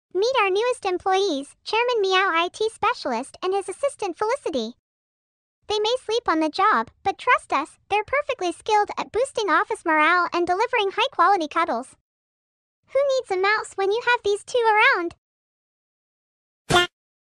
Working from home just got a whole lot cuter!
🖥 Working from home just got a whole lot cuter! From whiskers on keyboards to unexpected naps on important documents, these furry coworkers add a touch of purrfection to our workday.

😻 Whether they're offering "pawsitive" vibes during those Zoom meetings or reminding you to take a break with their adorable antics, our feline friends know how to make the "claw-some" balance between work and play.